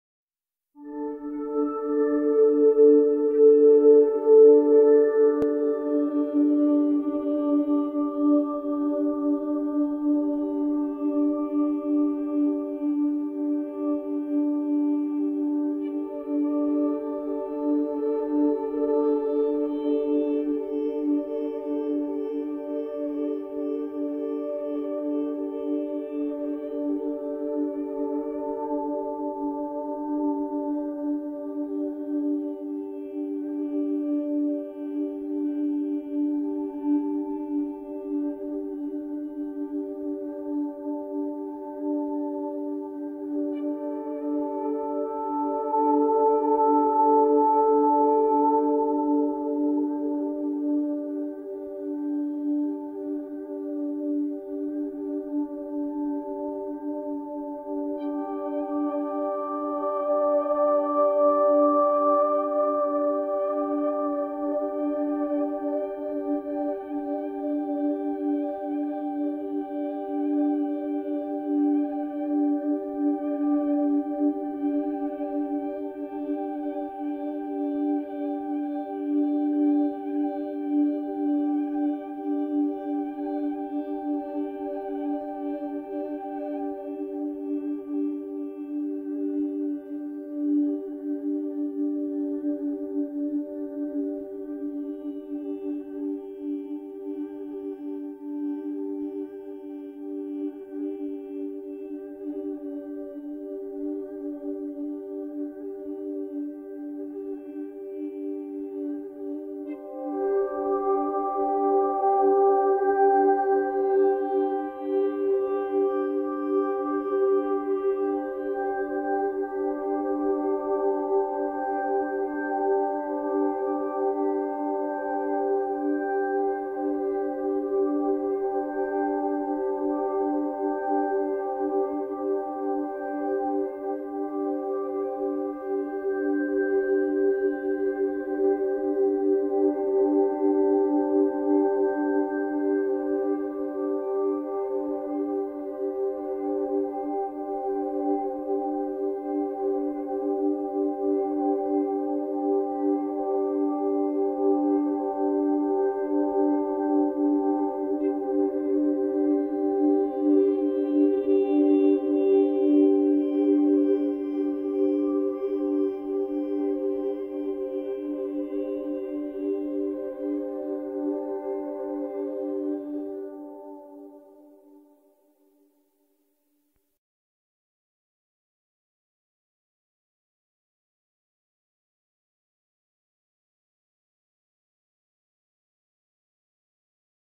Isolated Events: A Fractal Animation
3D fractal animation made with Mandelbulb 3D.